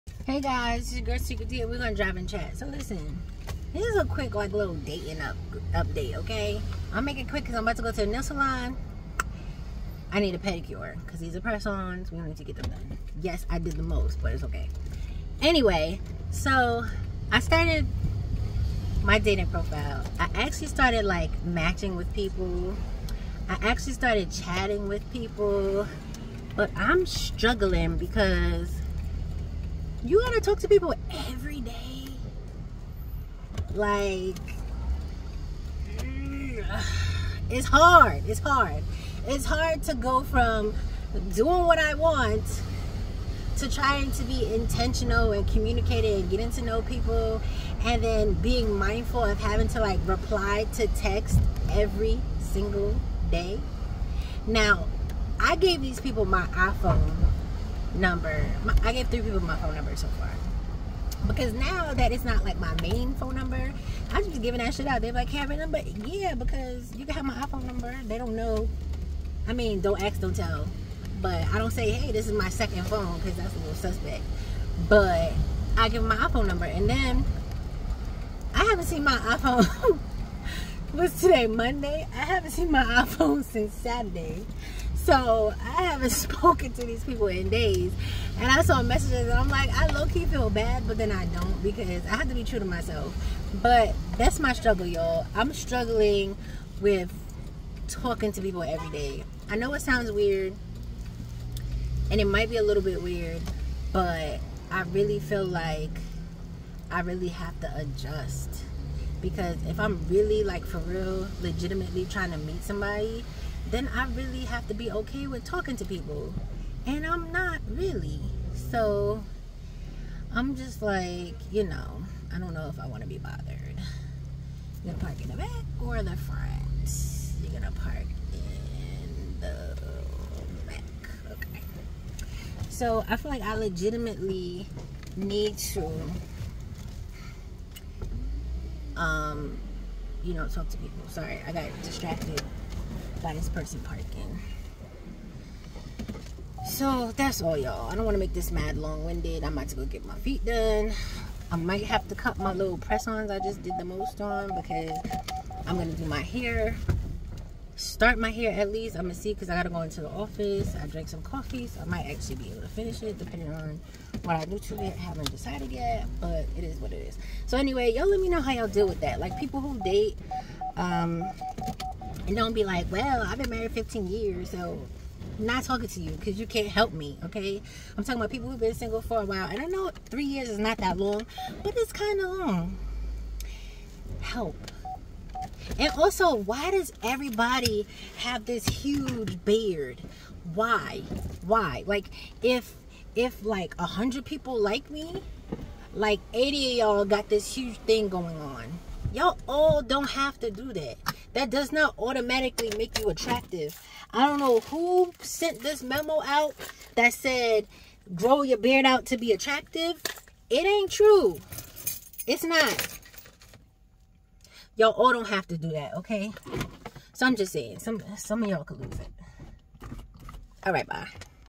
My dating life update/mini rant and cry for help 😫😫🤣🤣 Is it just me??? Does anybody have advice??
IG: SeekretD,  Seekrets_Corner,  and SeekretTheModel
Twitter and TikTok : SeekretD
Facebook pages: Seekret D and Seekret's Corner (store)